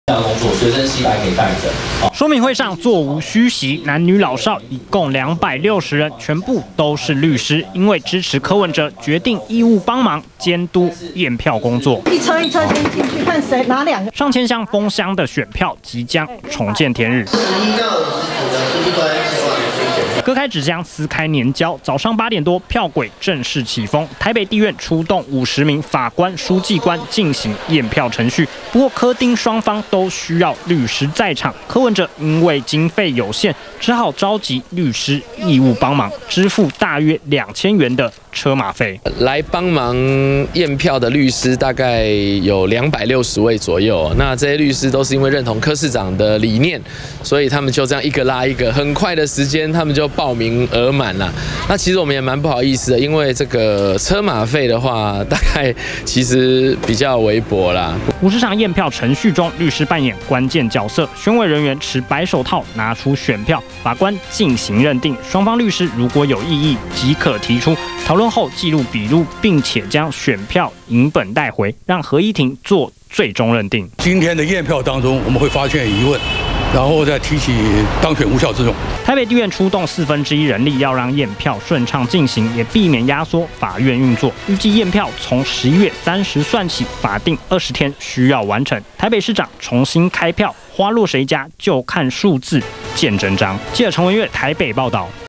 20181204中天新聞 「經費有限」柯P召集義務律師 260人到場支援
台北市長選舉，國民黨候選人丁守中，聲請重新驗票，不過總計50組的驗票配置，雙方每天都需要50名以上的律師在場，柯文哲對外表示經費不足，因此召集義務律師幫忙，沒想到3號晚間，居然出現260位律師到場聲援，要幫他義務驗票。